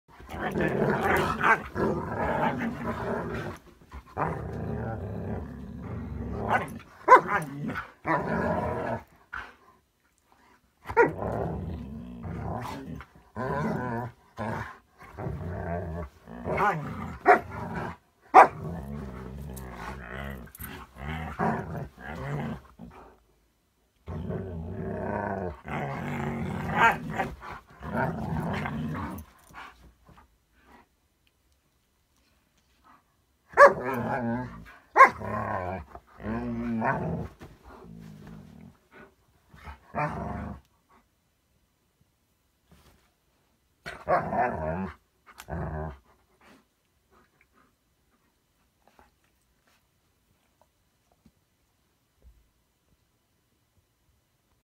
Husky wants sleep but German Shepherd has other plans
Kaden the German Shepherd just wants to play!